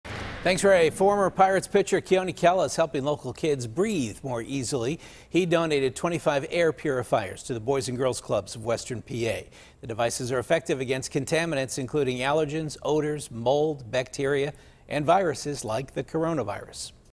Keone Kela Makes Donation
The former Pirates pitcher donated air purifiers to the Boys and Girls Club of Western Pennsylvania.